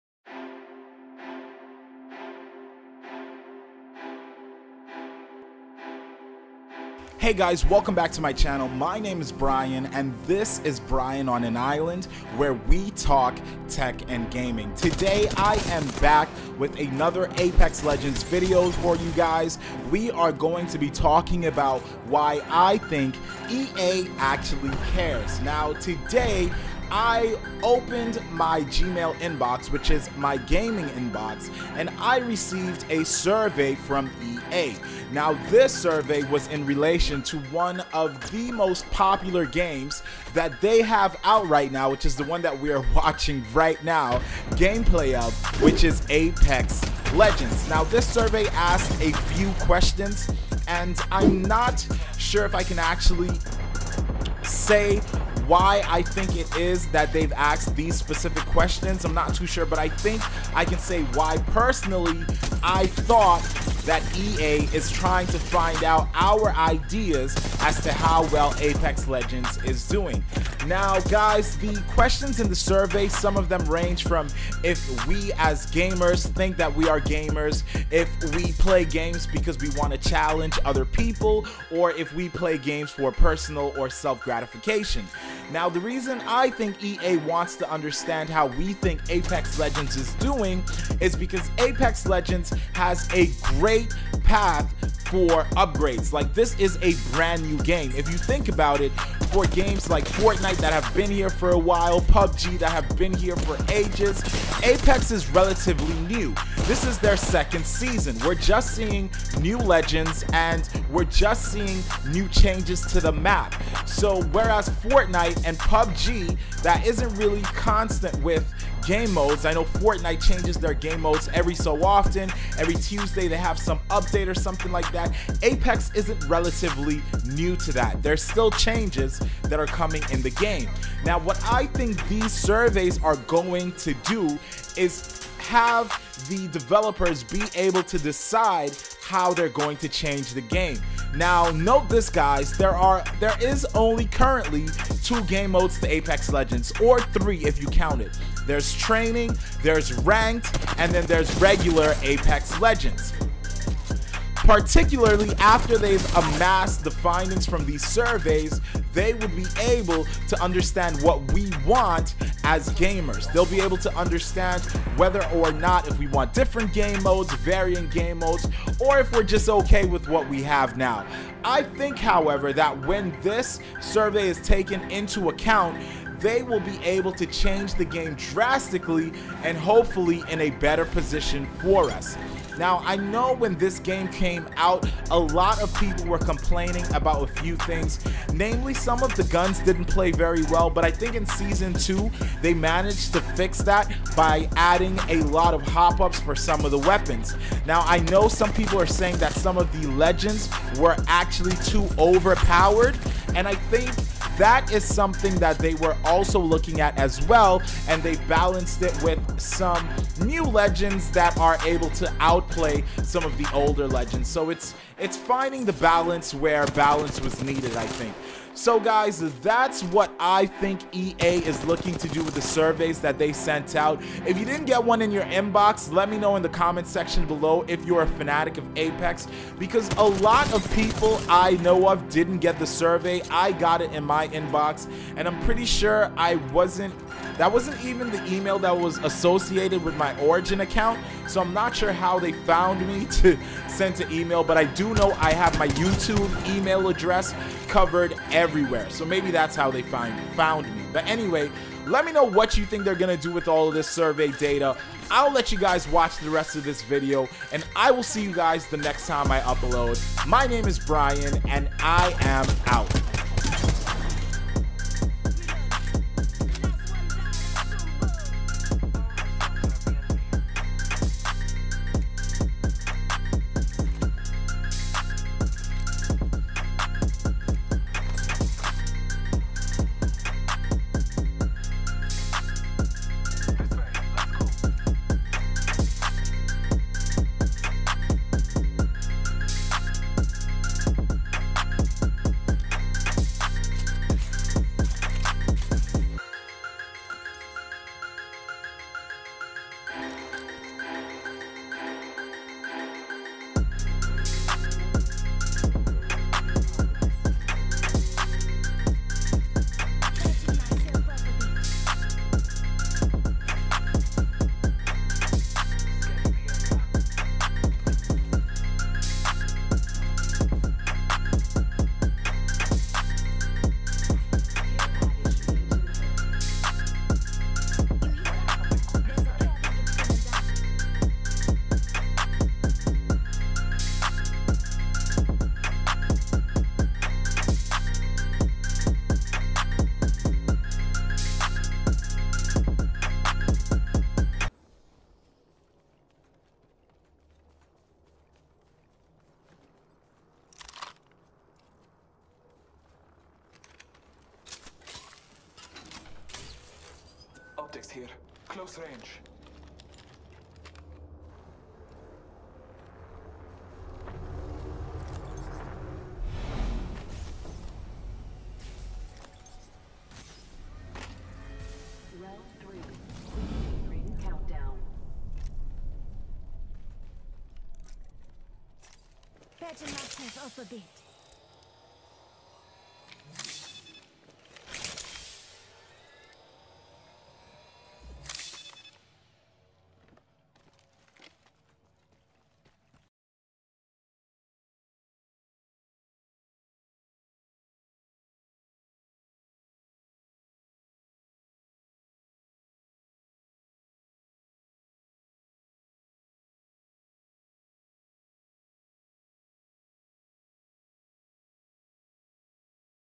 THIS IS WHY I THINK EA ACTUALLY CARES ABOUT APEX LEGENDS AND THE PEOPLE THAT PLAY IT
►My Equipment:

*CAMERA : Canon EOS M10 BLACK
* Mic(Neewer) Professional Studio
*USB Cables ( Top Grade Pro Cables)
*Small gorillapod
*Big Gorillapod
* Basic Tripod